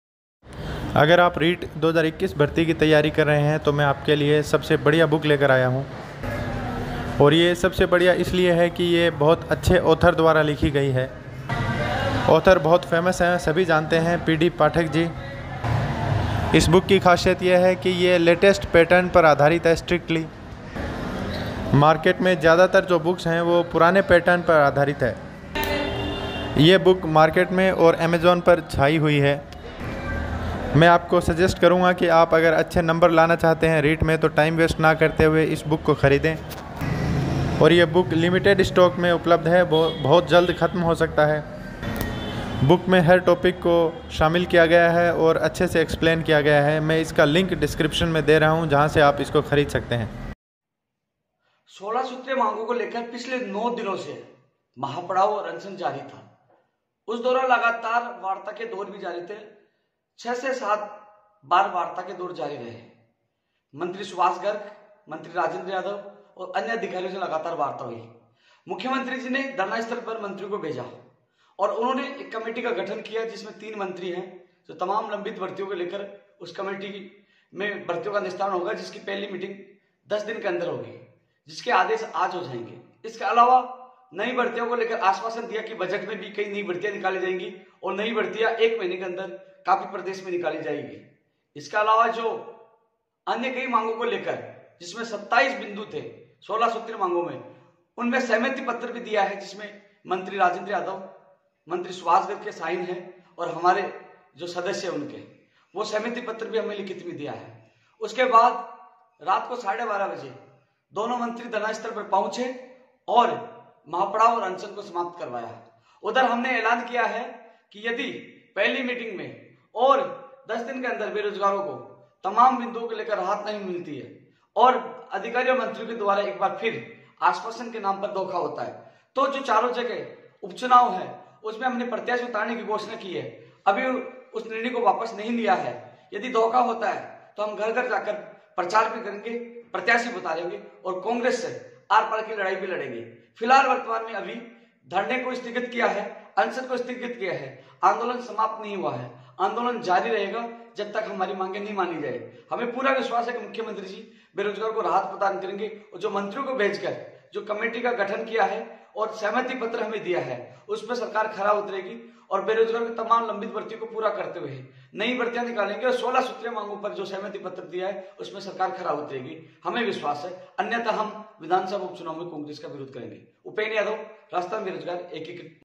बेरोजगारो की बड़ी माँगे पूरी हुई rpsc 1st grade, reet, librarian, police, ldc
लिंक किताब खरीदने का